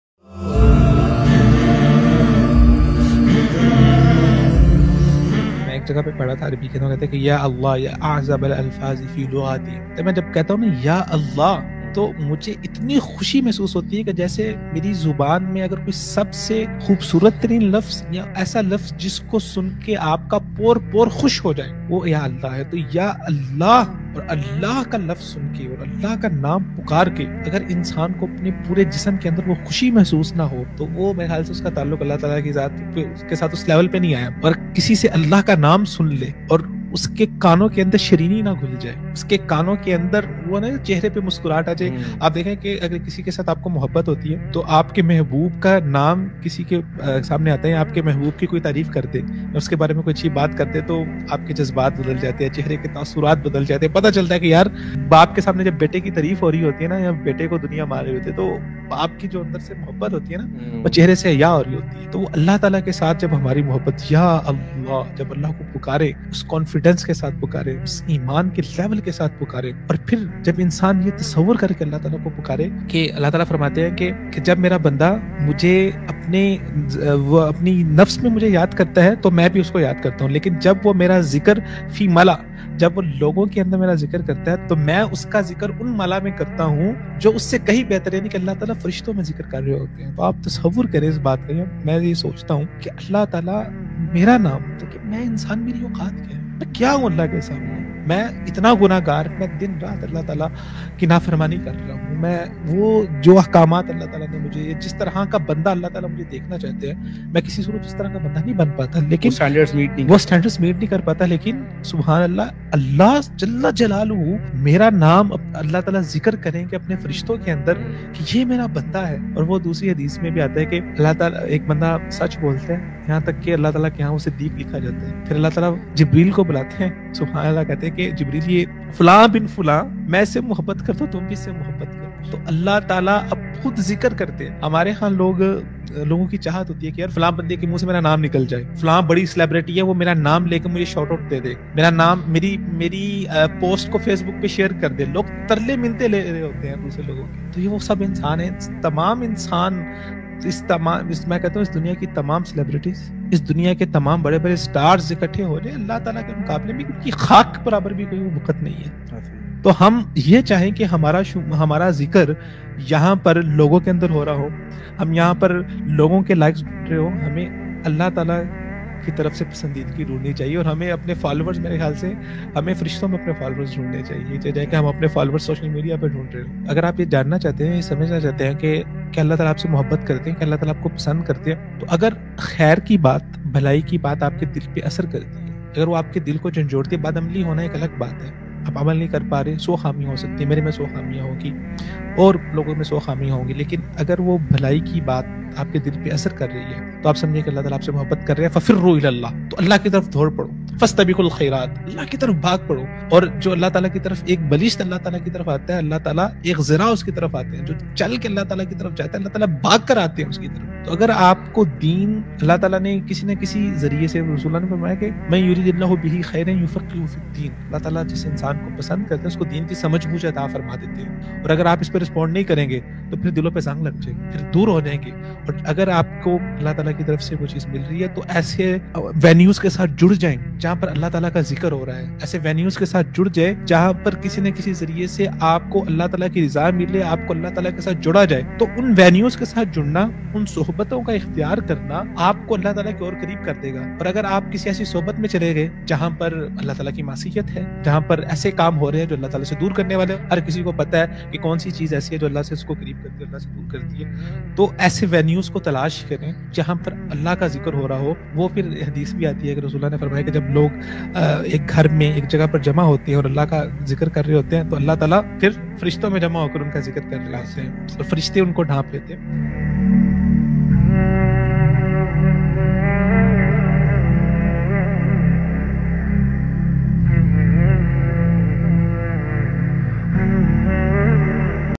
ALLAH Ki Mohabbat Kaise Paida Hoti Hai? Islamic Reminder by Sh Ubaid Ur Rehman| Al-Midrar Institute
ALLAH Ki Mohabbat Kaise Paida Hoti Hai ?
Allah ki mohabbat kaise paye ?
Allah ki mohabbat kaise hasil kare ?